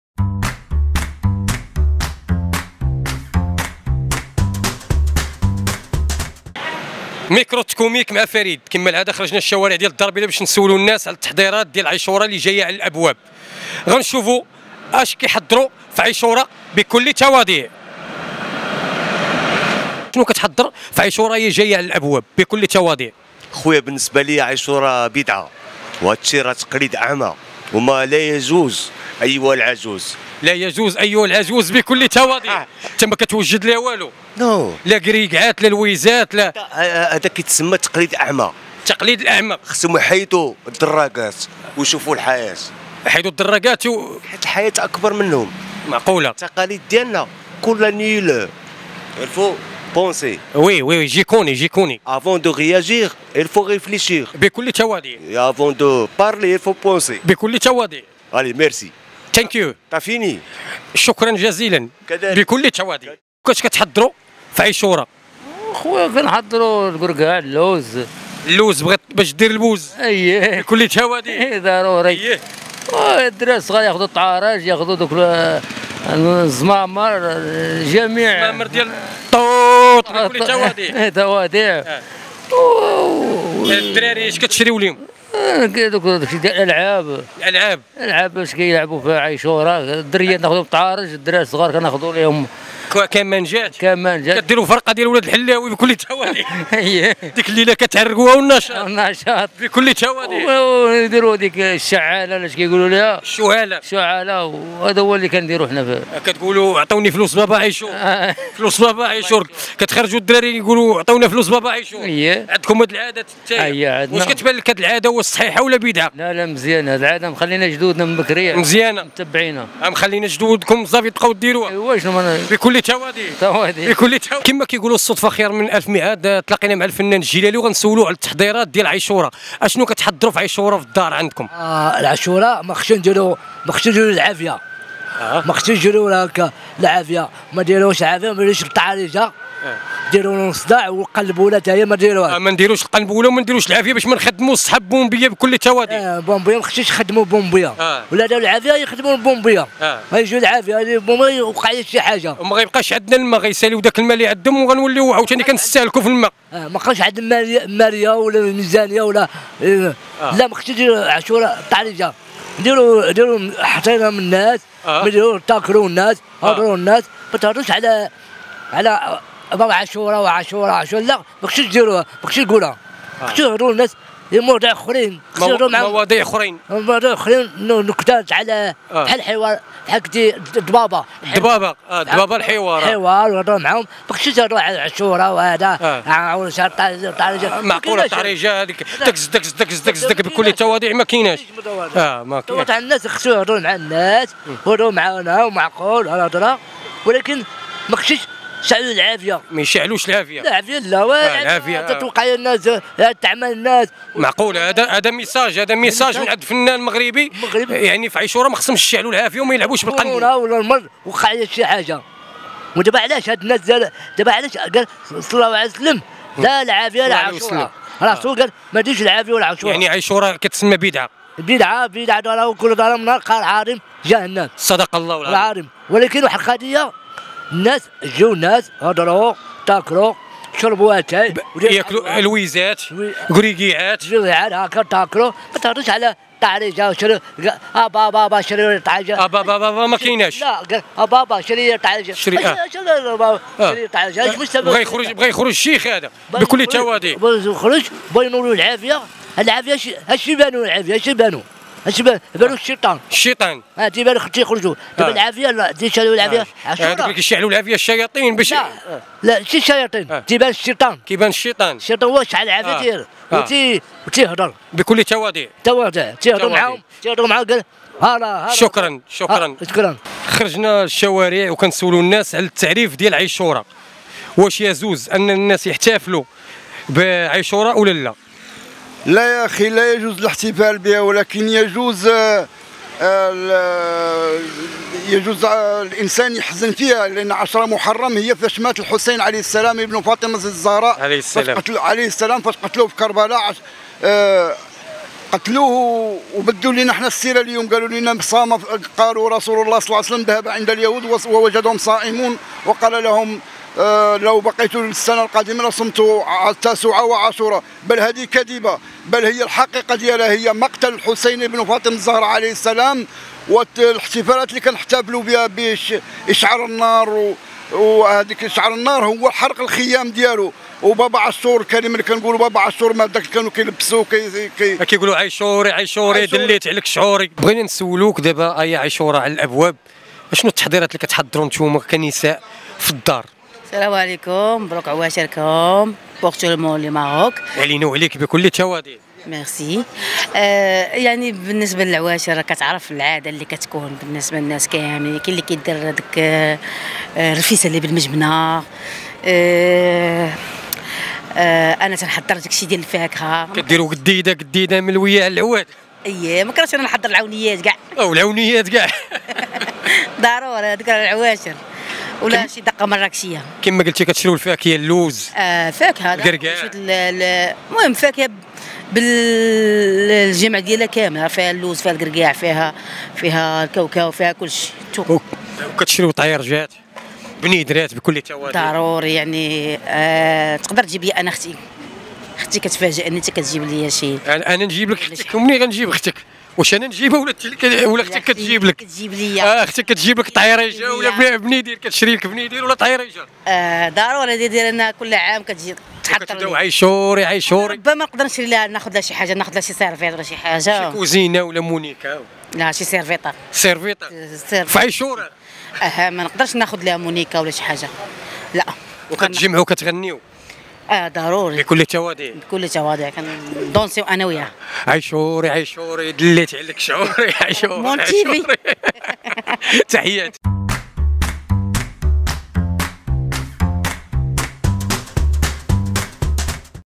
التكوميك مع فريد: تعرفوا على تحضيرات المغاربة في عاشوراء
ردود المغاربة  على تحضيراتهم بمناسبة  عاشوراء في ميكرو التكوميك مع فريد

# أخبار_عاجلة